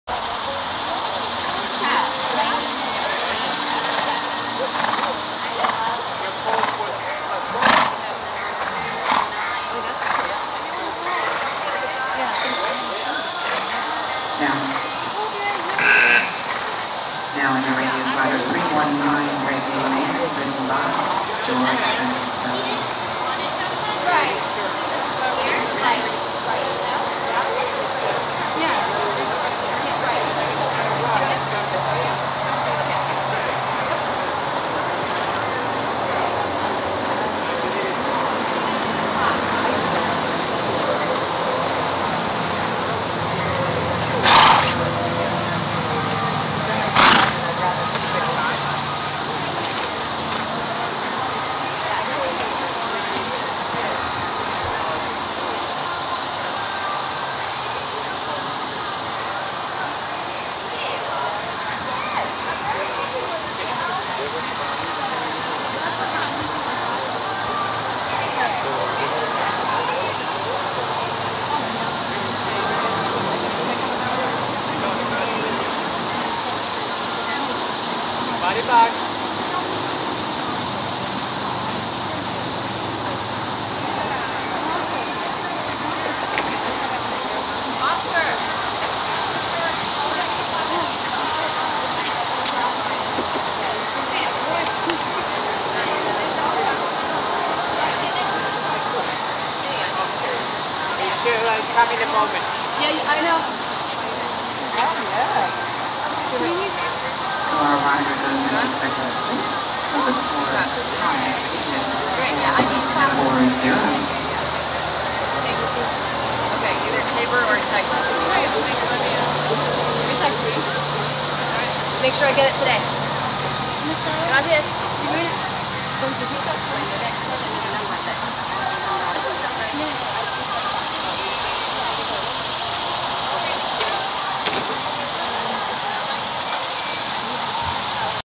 George Ventricelli and Regae Man Round 1
George Ventricelli and Regae Man at Palermo Show Stables in Bedminster, NJ on Sunday August 11, 2013.  Filmed by Focus Films, LLC of Parsippany, NJ